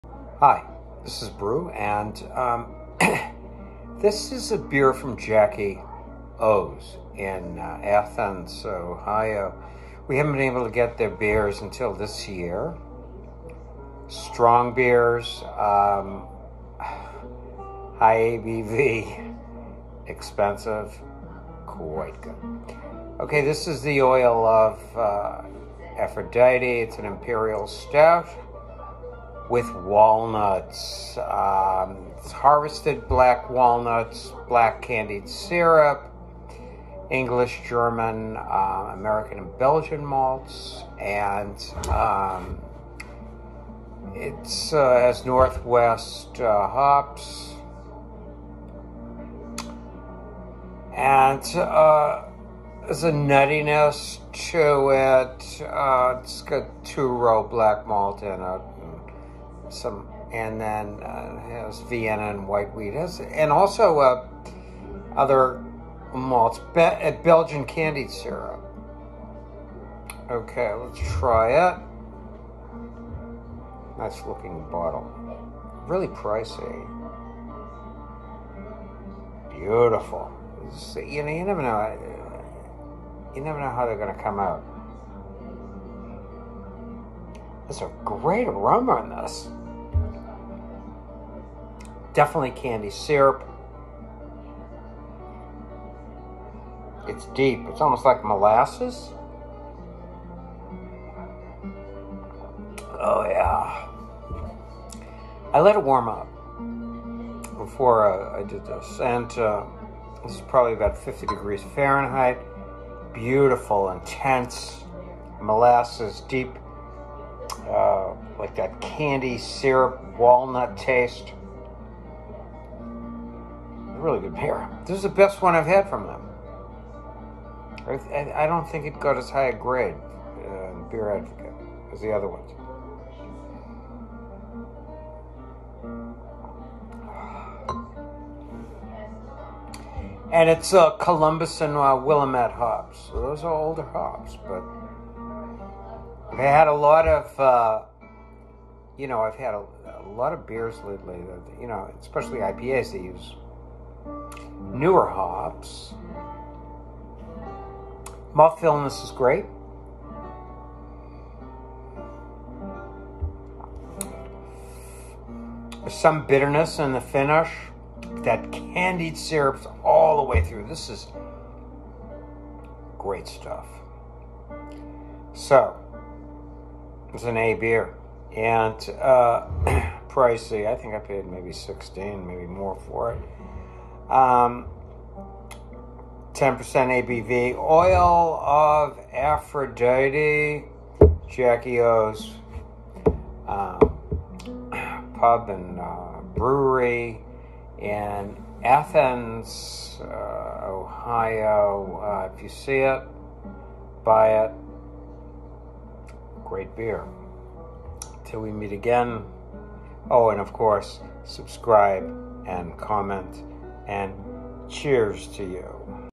Jackie O’s Oil of Aphrodite Double
Another deep, complex  beer from Jackie O’s Pub and brewery in Athens, Ohio. Belgian candy sugar and dark walnuts dominate the taste. Great beer!￼